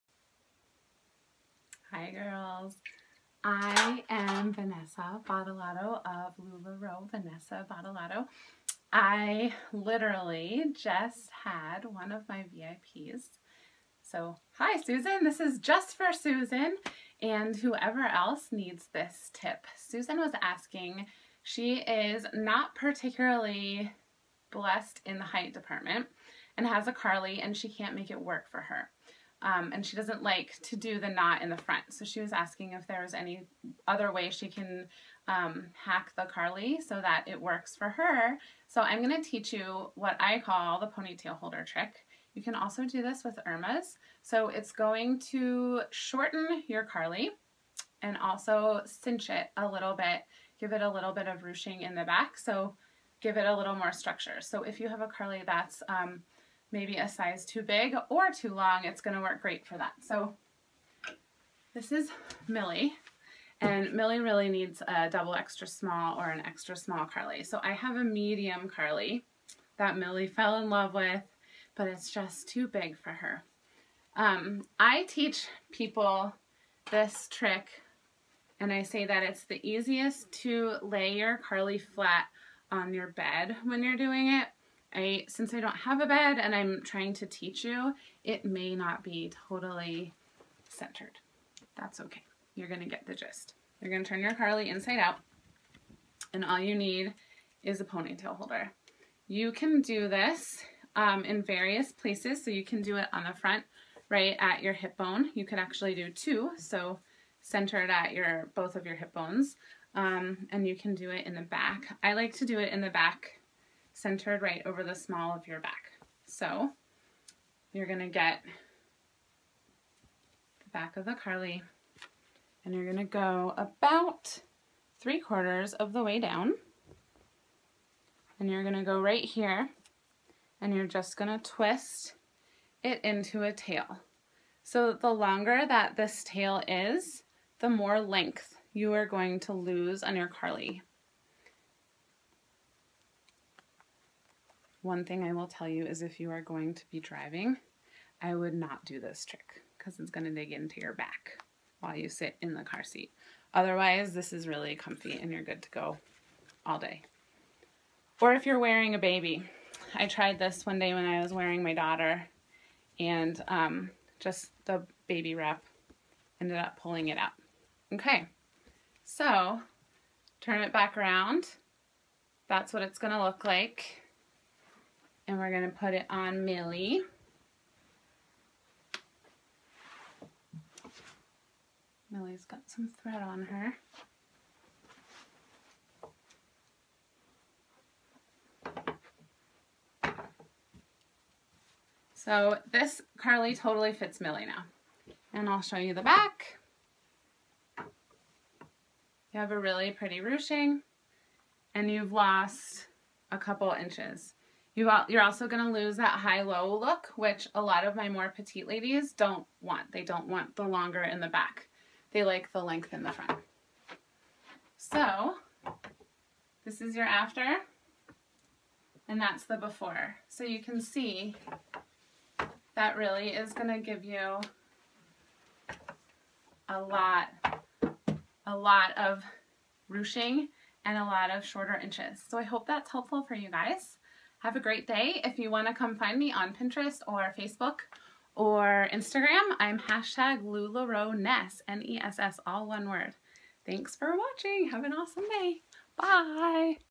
How to cinch and shorten your Carly!
Have a Carly that's too big? I can help.